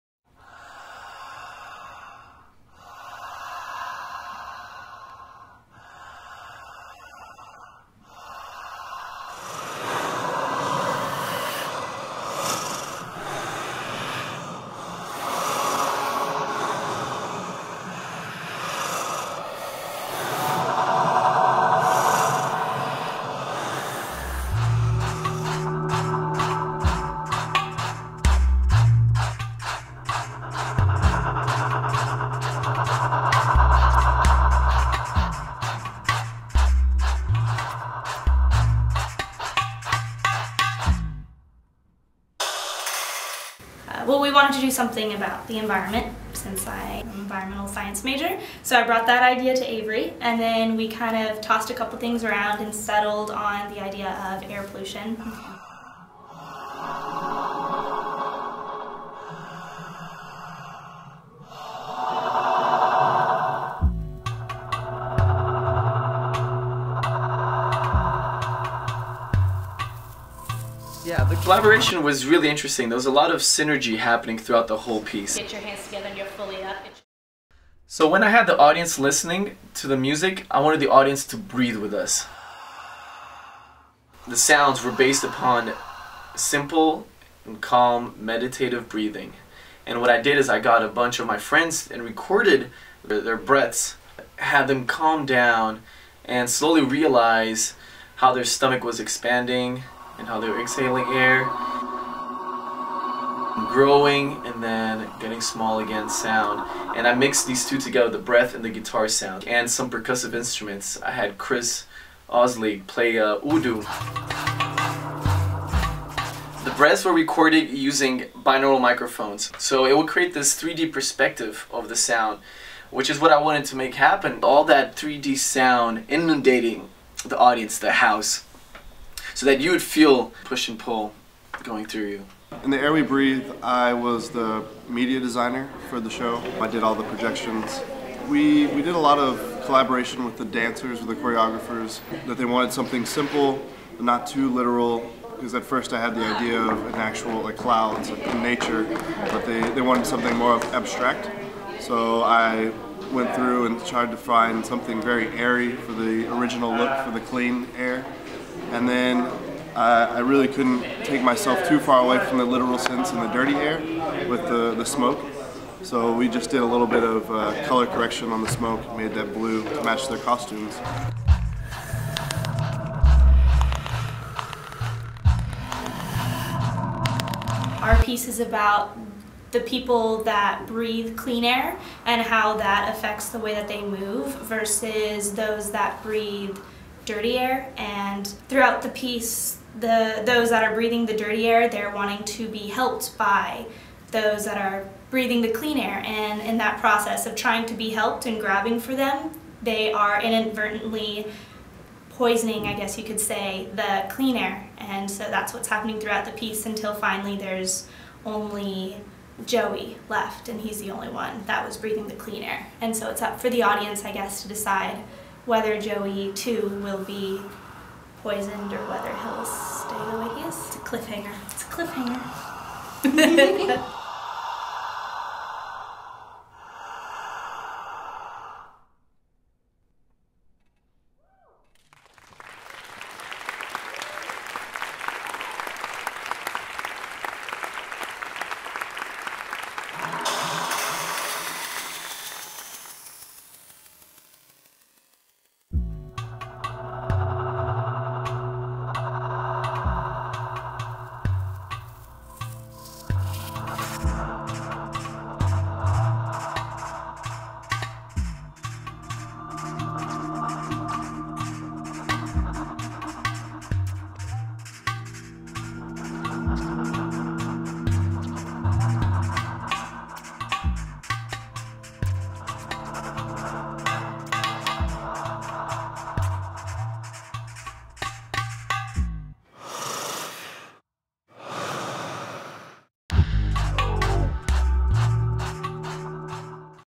Making-of "The Air We Breathe"
Behind the scenes of the dance piece  "The Air We Breathe".

Choreographers:
Avery Elizabeth Hurst and Lily Amelie Hayes

Dancers
Ema Watanabe, Rebecca Bagley, Joey Gaona, Kelsey Oliver and Hillary Richerson

Composer
Yago de Quay

Video Designer
Anthony Guillen

Costume Designer
Kelly Ruiz

Videographer/Editor
Meg Seidel